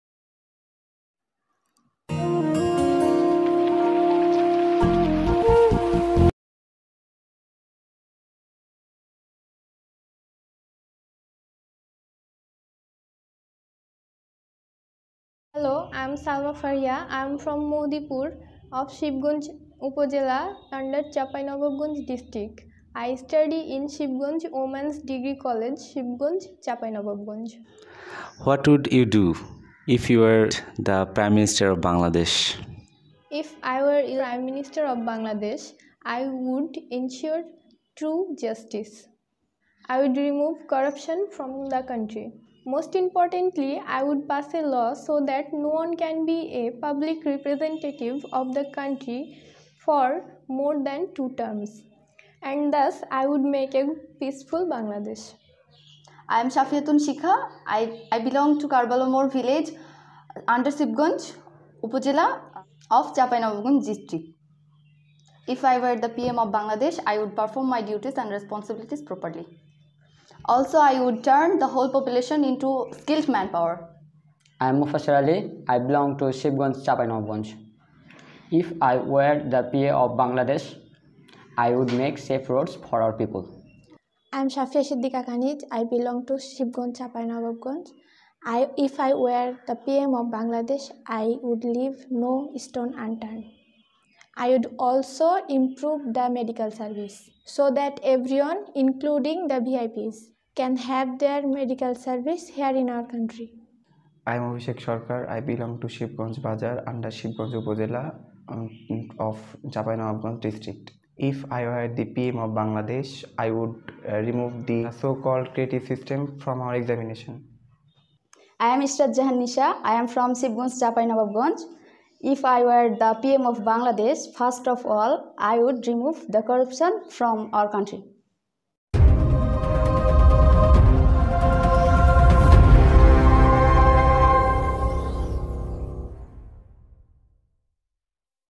If I were the PM of Bangladesh,...
Subject: If I were the PM of Bangladesh,...
Participants: Salma Faria, Safiyatun Shikha, Esrat Jahan Nisha, Abhishekh, Mofasser & Kaniz.
Videography & Editing: Mostafa Mamun